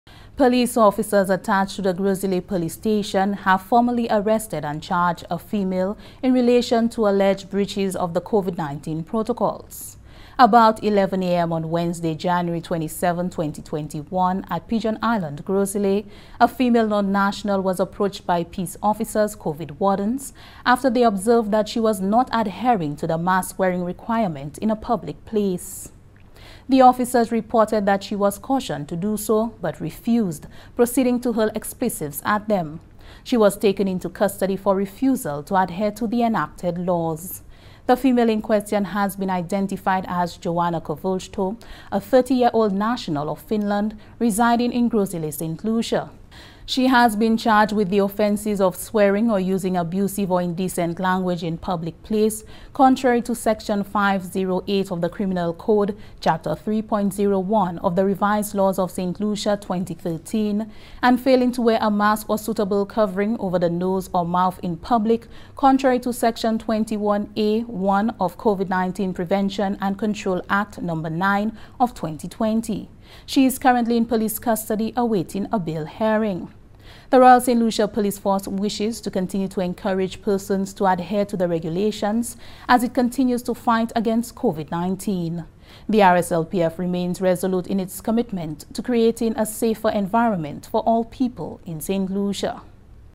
Female charged for COVID-19 protocol breach
Police Officers attached to the Gros Islet Police Station have formally arrested and charged a female in relation to alleged breaches of the COVID-19 protocols.
Johanna Koivulchto, a 30-year-old national of Finland, residing at Gros Islet, was taken into custody for the refusal to adhere to the enacted laws, and has been charged with the offences of Swearing or Using Abusive or Indecent Language in Public Place (Contrary to Section 508 of the Criminal Code Chapter 3.01 of the Revised Laws of Saint Lucia 2013) and Failing to Wear a Mask or Suitable Covering Over the Nose or Mouth in Public (Contrary to Section 21 (a) (1) of the COVID-19 (Prevention and Control) Act No. 9 of 2020. She is currently in police custody awaiting a bail hearing.